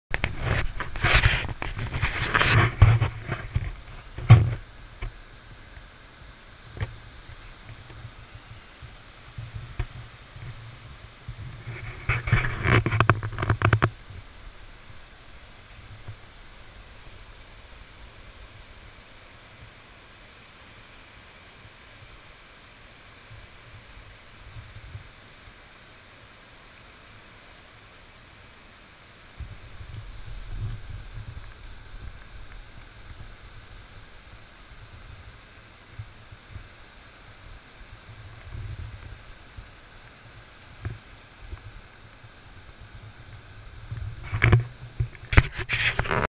Laureen Dudit - Championnat du monde Surfcasting - Combat avec une raie guitare de 85cm
Laureen Dudit en plein combat avec une raie guitare de 85 cm lors du championnat du monde de surfcasting en novembre 2017.